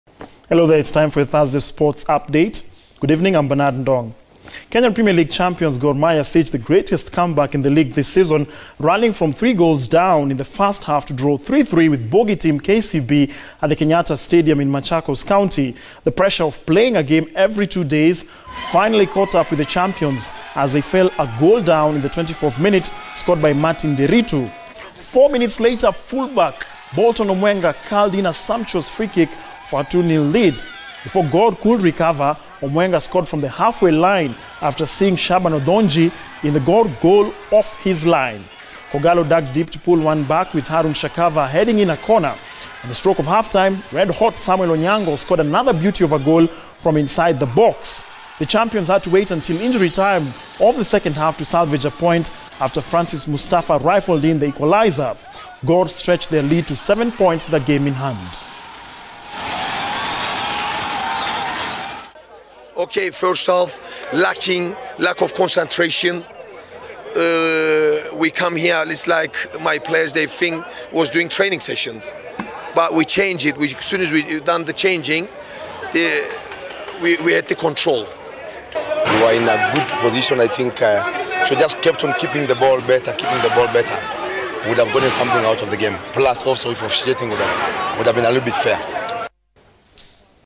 KPL |Gor Mahia stage epic comeback
Kenyan premier league champions Gor Mahia staged the greatest comeback in the league this season, rallying from 3 goals down in the first half to draw 3-3 with bogey team KCB at the Kenyatta stadium in Machakos County